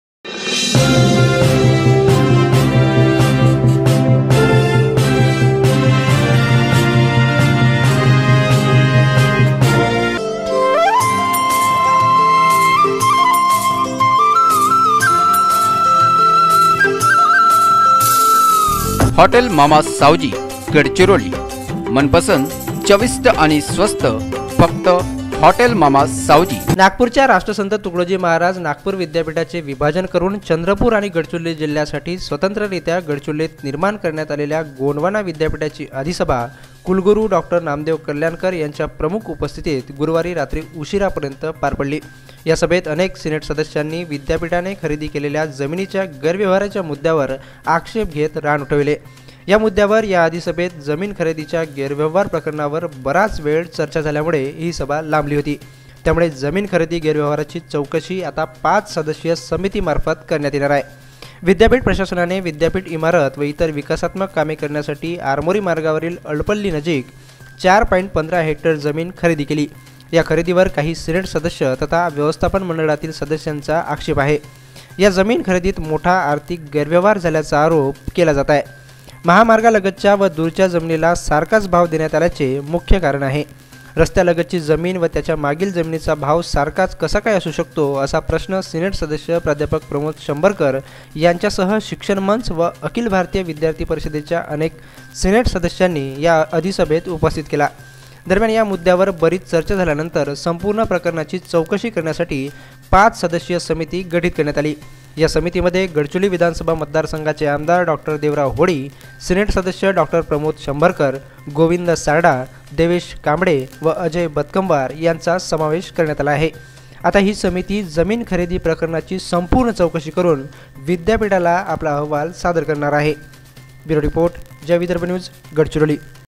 गोंडवाना विद्यापीठाच्या जमीन खरेदी व्यवहाराची होणार चैकशी
पाच सदस्यीय समितीचे गठण, अधिसभेत गाजला मुद्दा